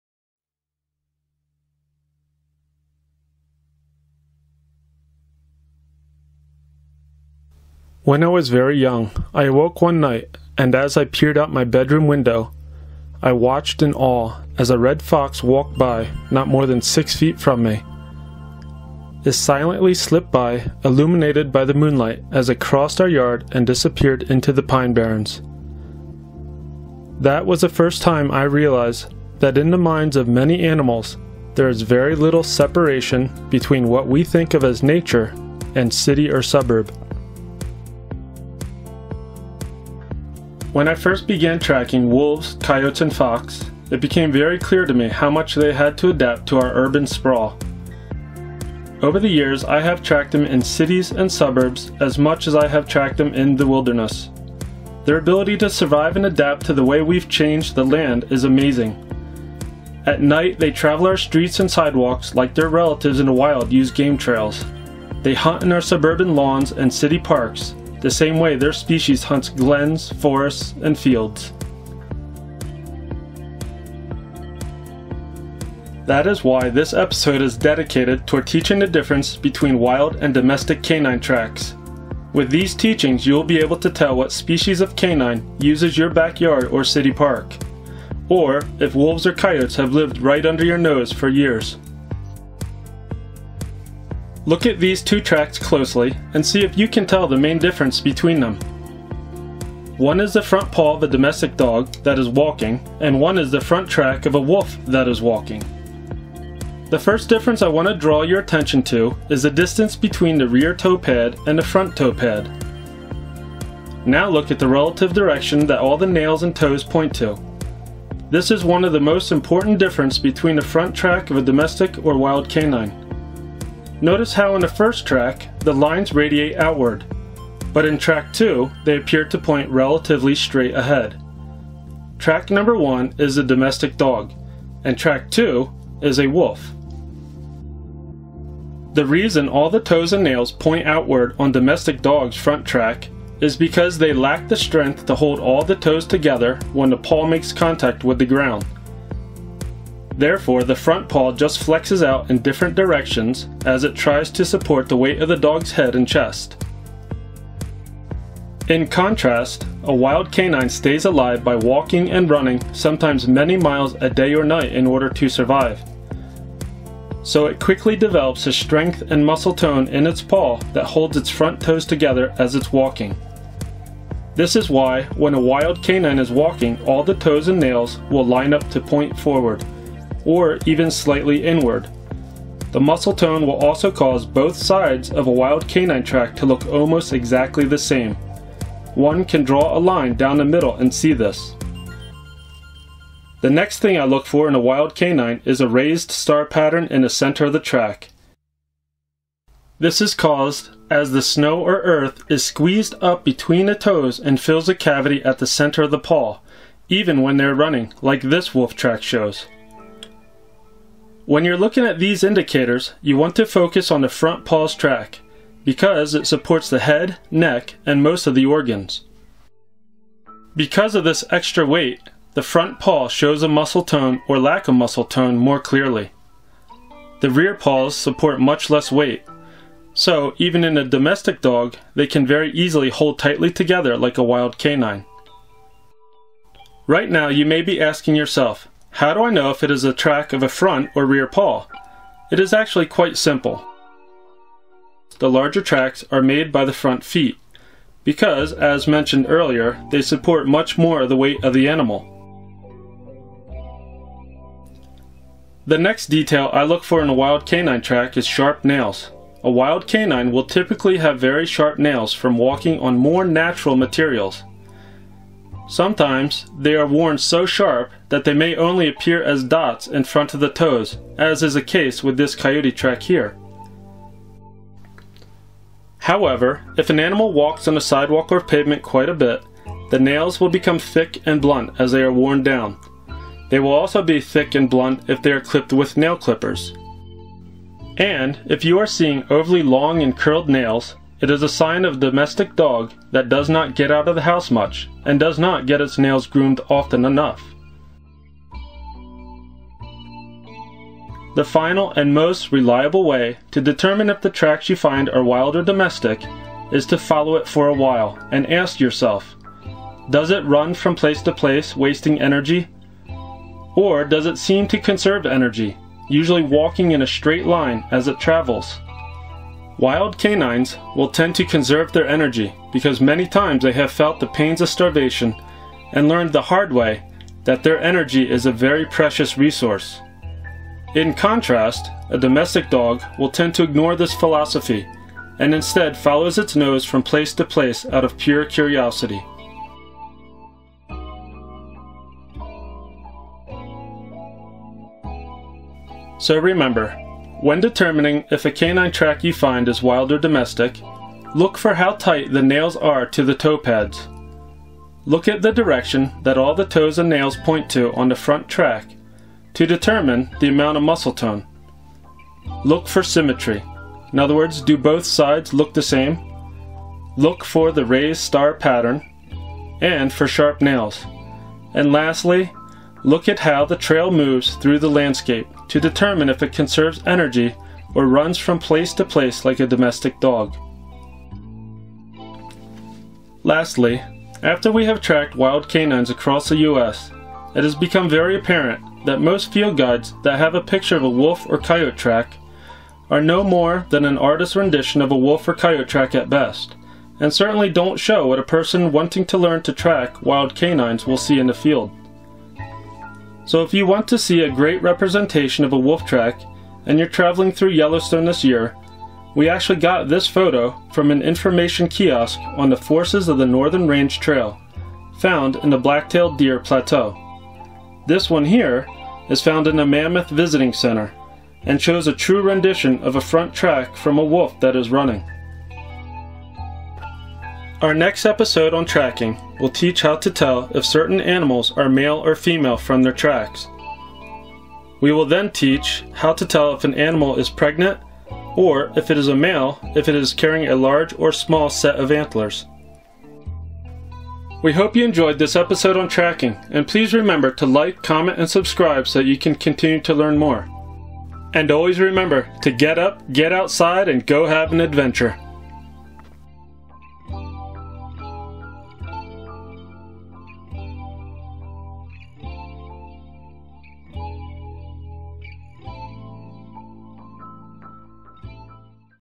Epic Animal Tracking Episode #1: Wolf, Coyote, Fox, Domestic Dog - Identification Tips
Tracking: Wild vs Domestic Canine Tracks
One of the most common questions we get about animal tracking is from people simply trying to find out if the track they found is from a wild or domestic canine. We have found over our years of tracking instruction that learning this distinction is the best place to start tracking, as it is the foundation for opening our eyes to the finer details that come later.

For the next several episodes of Into Their World, we will be taking you on an exploration of how to read what animals have left behind. We will show you how to identify tracks, scat, and sign of common animals in the United States. But it's more than just classifying the tracks, it's about an animal being hungry, pregnant, wounded, a member of a group, predator or prey, and so much more. Please join us on this journey!

All video content filmed and edited by Nature Tech Family.
Photos by Nature Tech Family or sourced from Pixabay.com and Commons.wikimedia.org.